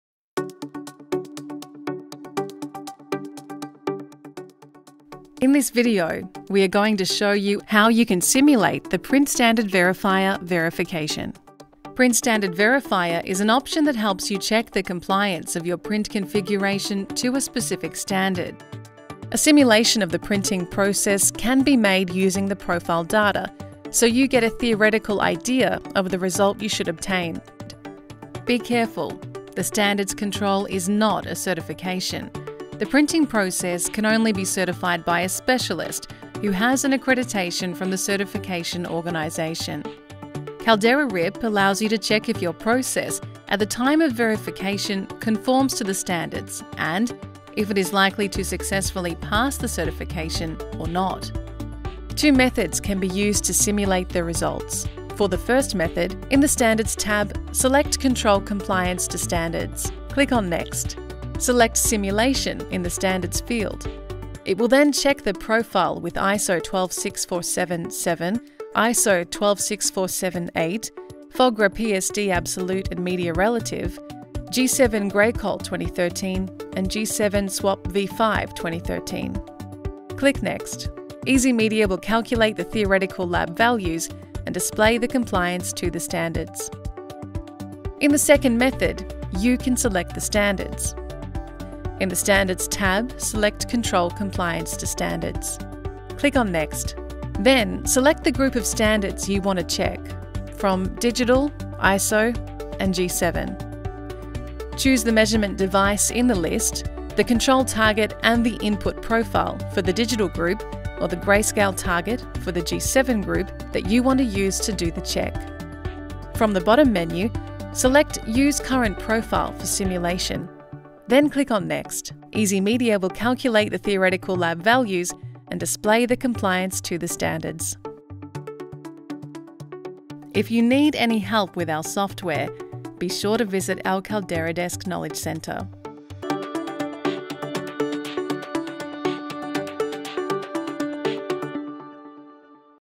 Tutorials CalderaRIP : HowTo Simulate the PrintStandardVerifier verification
In this Video we are going to show you "How you can simulate the PrintStandardVerifier verification". PrintStandardVerifier is an option that helps you check the compliance of your print configuration to a specific standard. A simulation of the printing process can be made using the profile data so you get a theoretical idea of the result you should obtain.
For any help with our software, be sure to visit our Knowledge center